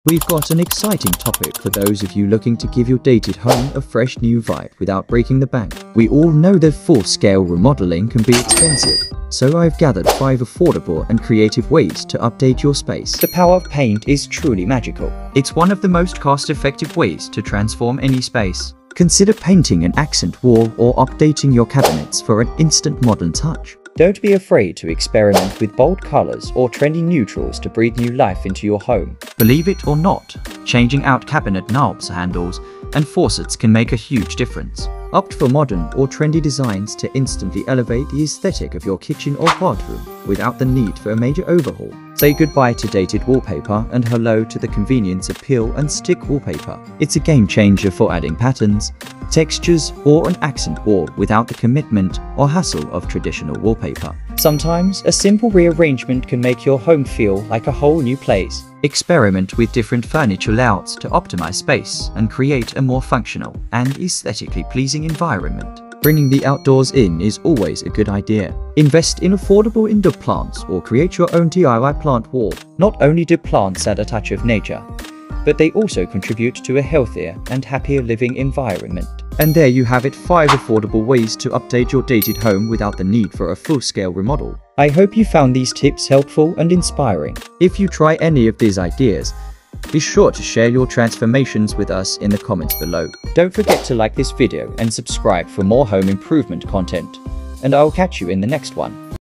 5 AFFORDABLE WAYS TO UPDATE A DATED HOME WITHOUT REMODELING
Ready to give your home a makeover without breaking the bank? In today's video, I've got you covered with 5 Affordable Ways to Update a Dated Home Without Remodeling! From the magic of paint to DIY wall art and budget-friendly finds, these tips will breathe new life into your space. 💡🌿 Don't let a tight budget stop you from creating a home you love. Watch now and get ready to transform your space on a dime! 🔨🎨